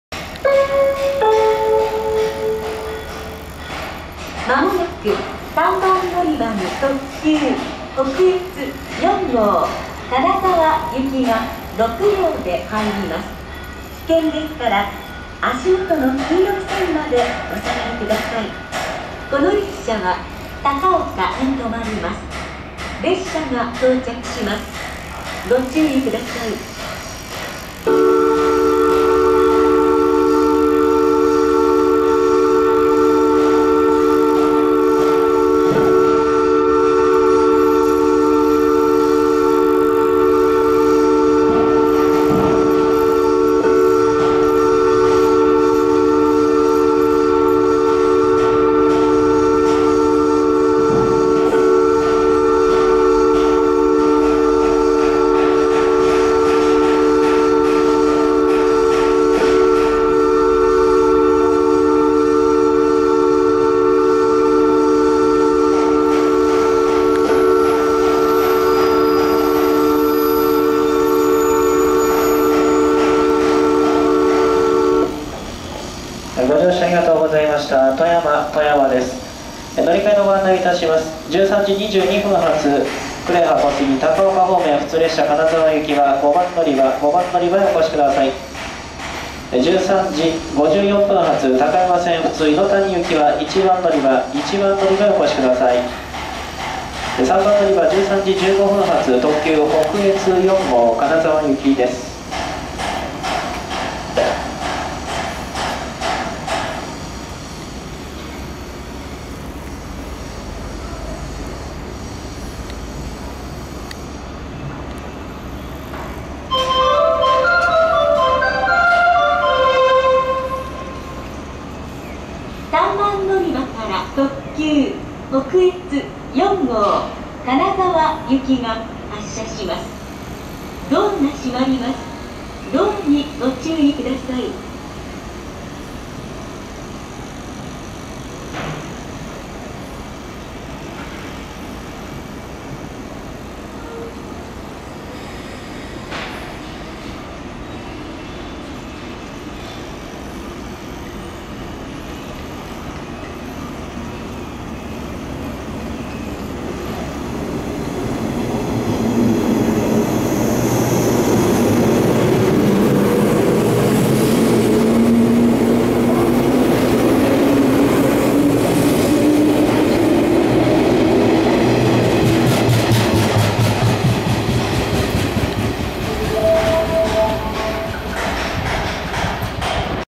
485系 北陸本線 特急北越4号 富山駅着発 （音声のみ）
諸事情で接近は音声のみです。
2012年10月24日録音